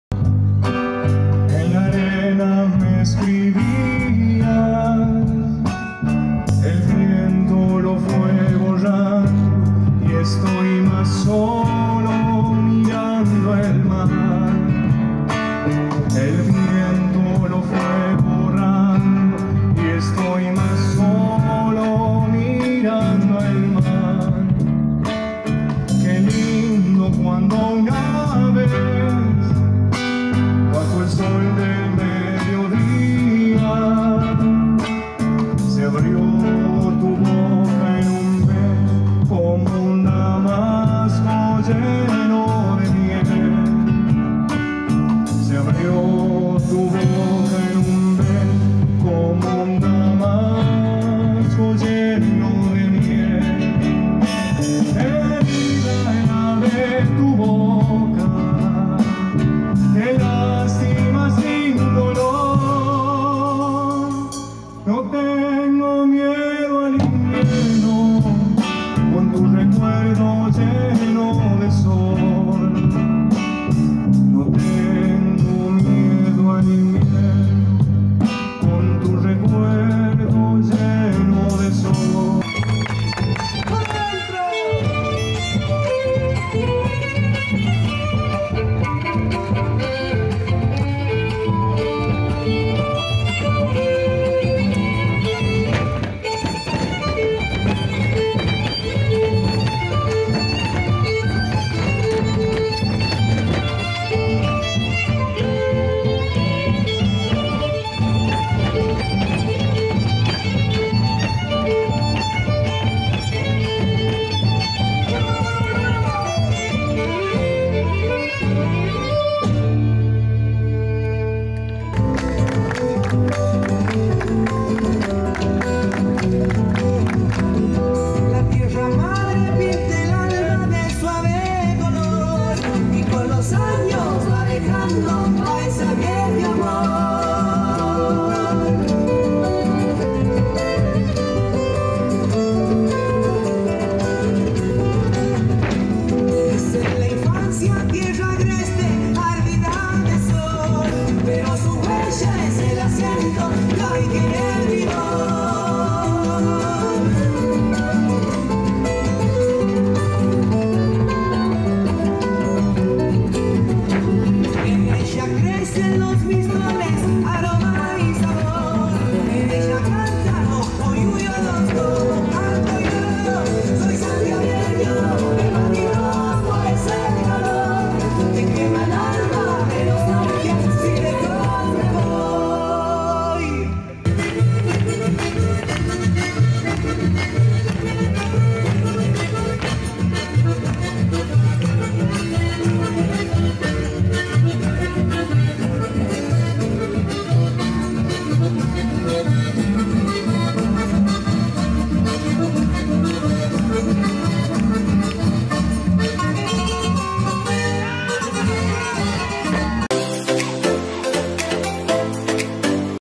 6 -125º ANIVERSARIO DE LAS GARZAS - SHOW ARTISTICO A LA TARDE NOCHE.-
6- 125º ANIVERSARIO DE LAS GARZAS - SHOW ARTISTICO A LA TARDE NOCHE, EL ESPECTACULO COMENZO CON LA PRESENTACION DE GIANFRANCO SANDRIGO " Y LUEGO EL BALLET YEROKI. LA MUSICA, LA DANZA Y EL CANTO EN TAN IMPORTANTE FECHA.-